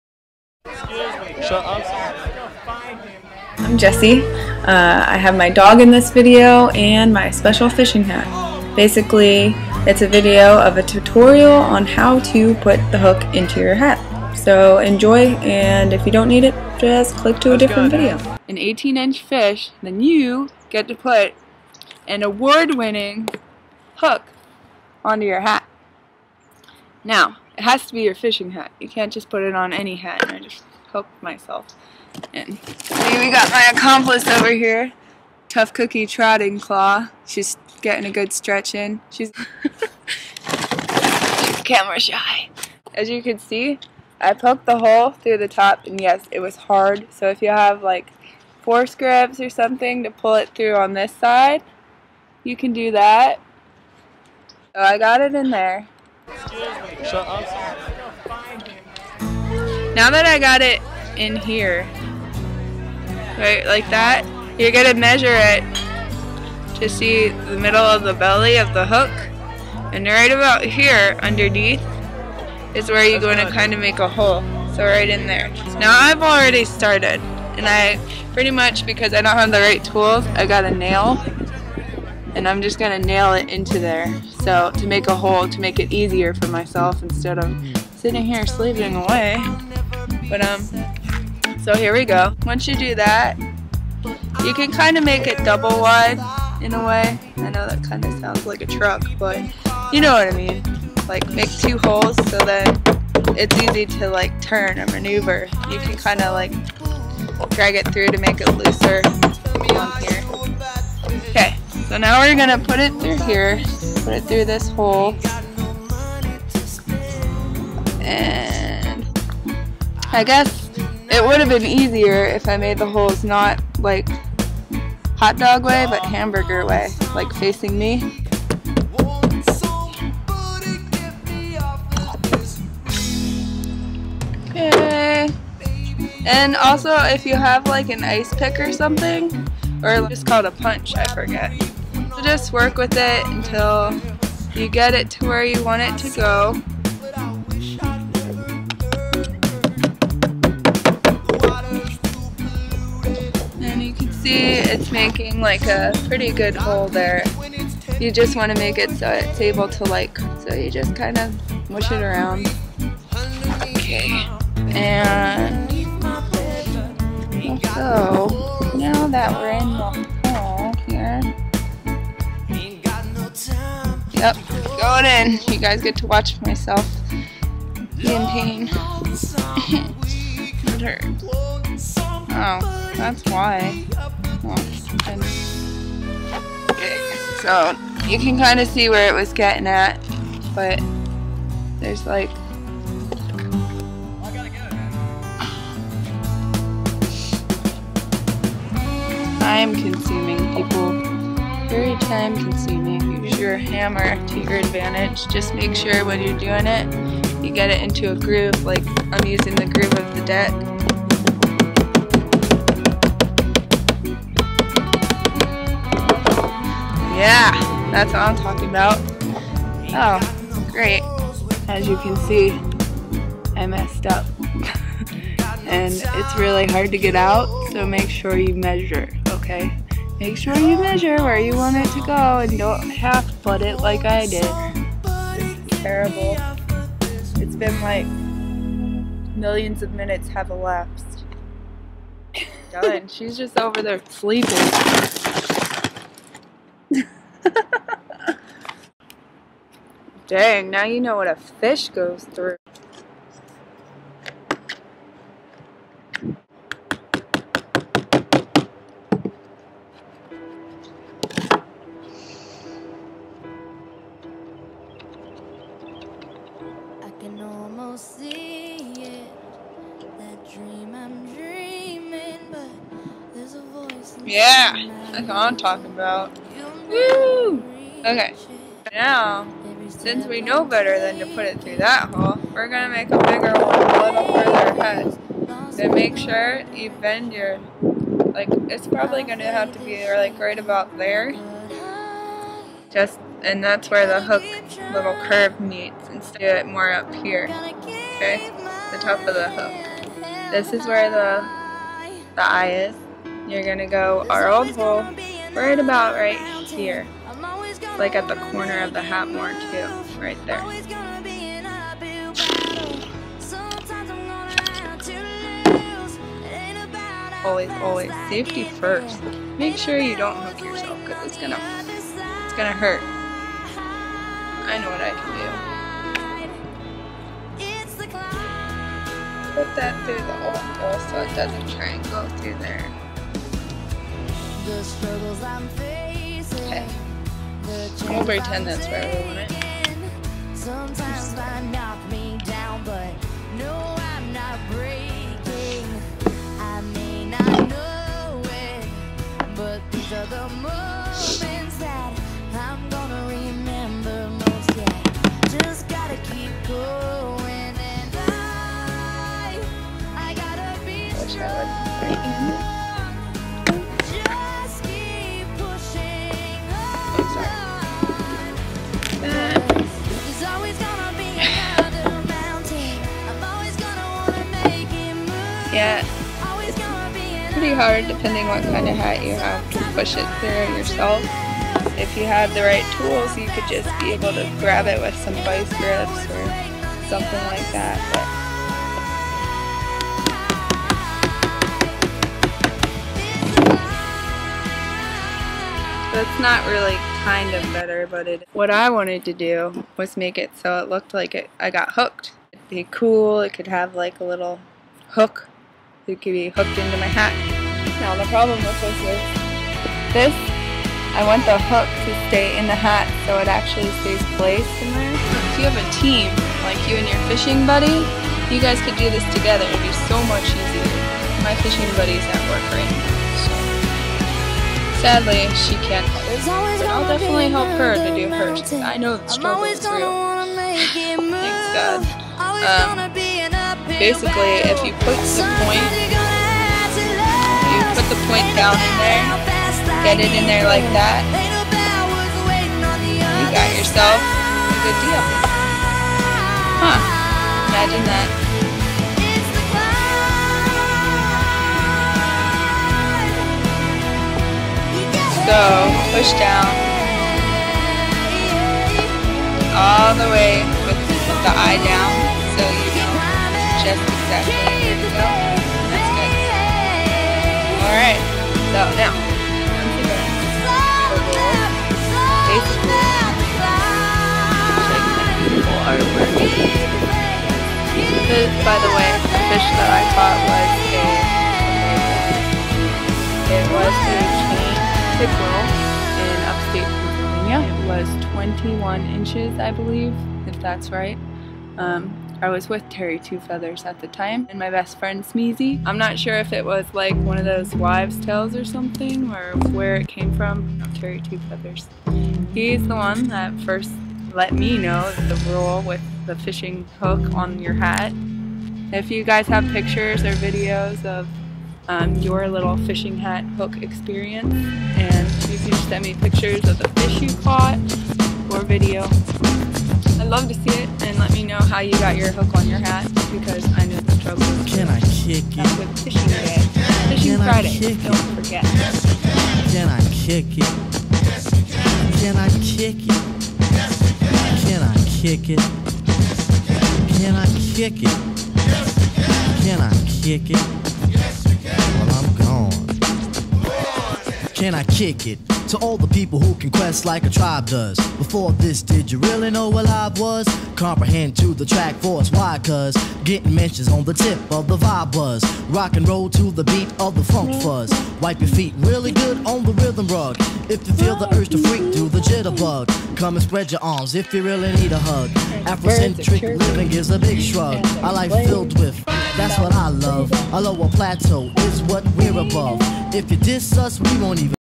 Fishing hat- how to put on the hook
Jessie teaches you  special way to get the fishing hook on your hat.  With speceal appearance from TC (tough cookie trotting claw).  Also includes Terry two feathers story.
fishing, fish, fishing hat, fun, do it yourself, summer, spring, fall, terry two feathers, trotting claw, catch and release